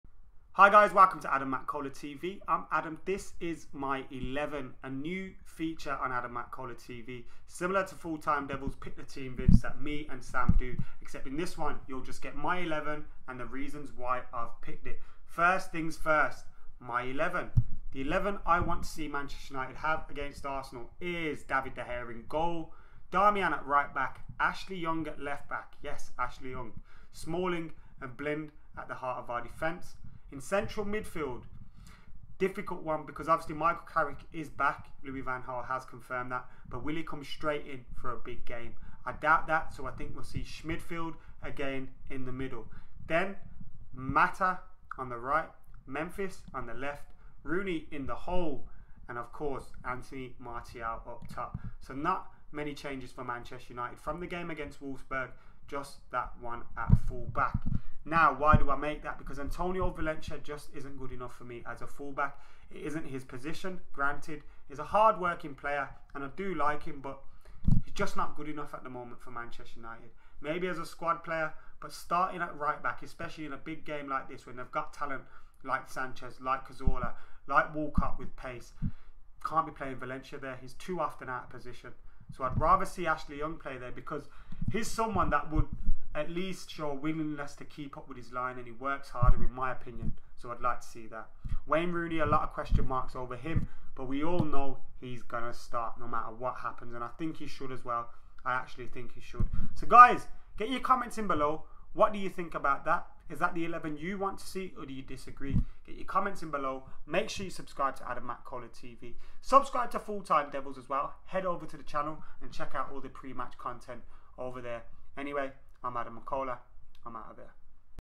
My XI: Arsenal v Manchester United | Predict The Team | 04.10.2015
Manchester United take on Arsenal this Sunday, who will feature in Louis van Gaal's XI? Get your predicted line-ups in!

Will Antonio Valencia remain at right back or will Ashley Young come in?

Michael Carrick is fit, does he start or will Louis stick with the Schmidfield?

Get involved below!

AdamMcKola.com